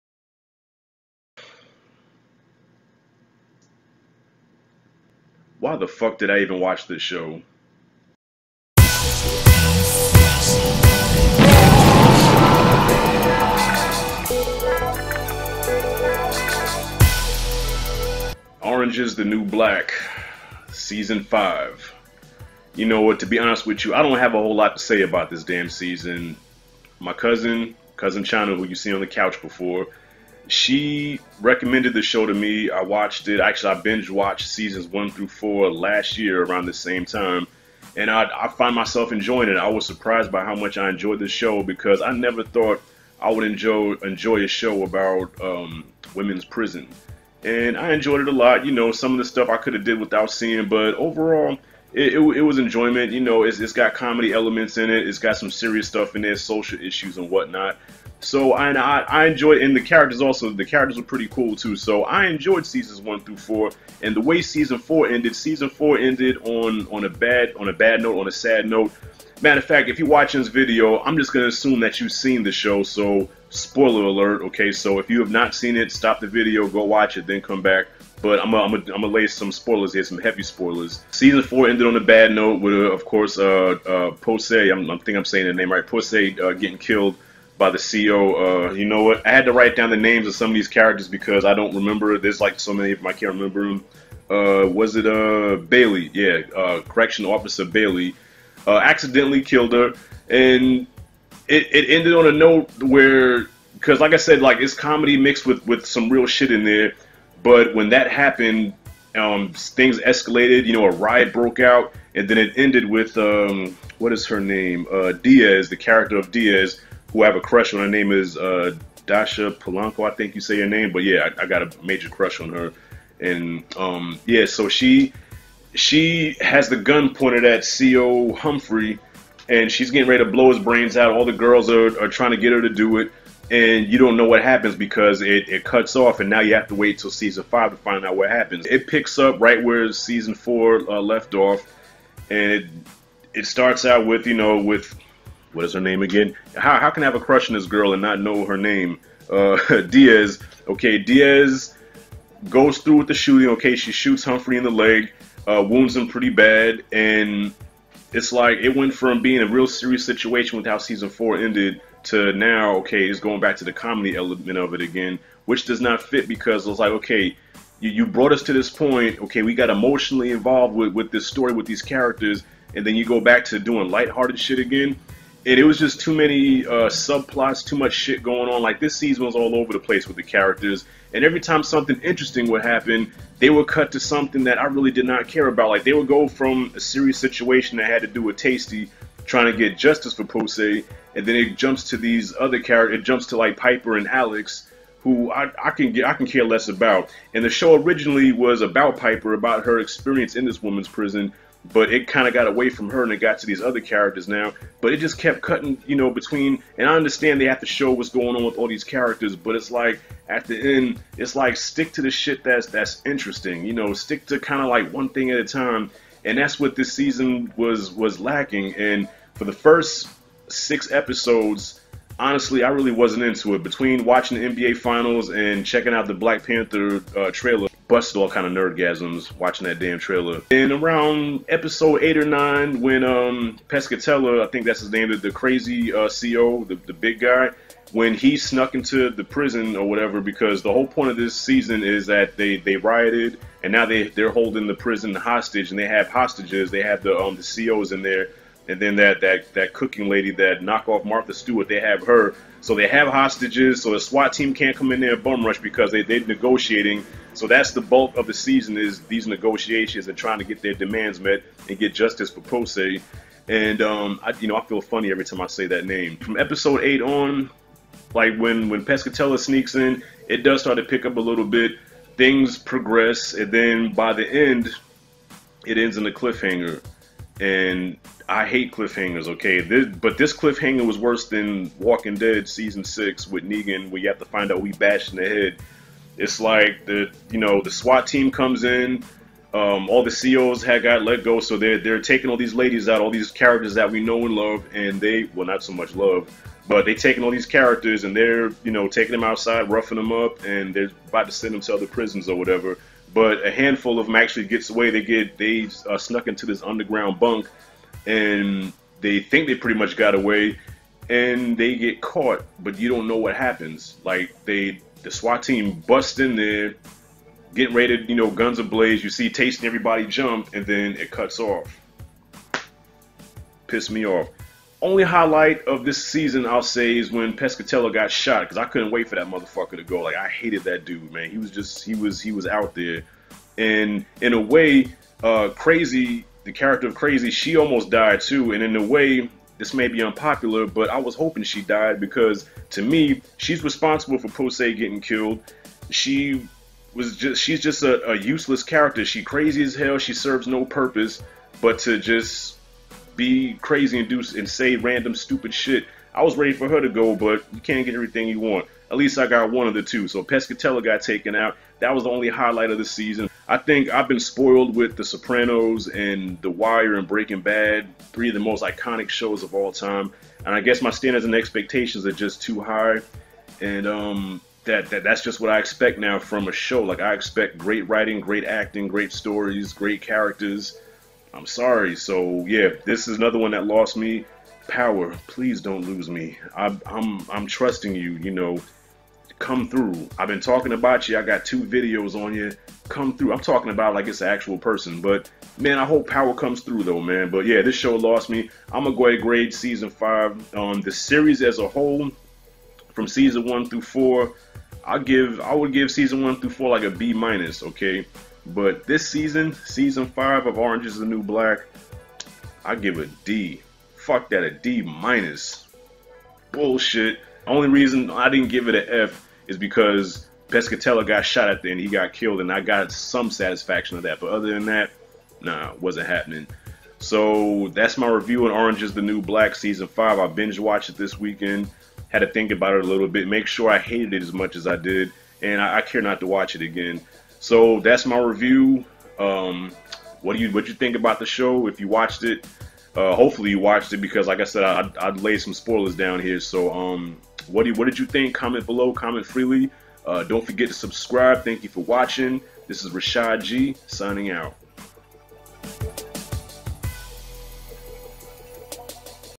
ORANGE IS THE NEW BLACK SEASON 5 Review ( Spoilers)
My review of Season 5 of Netflix's hit series, Orange is the new Black.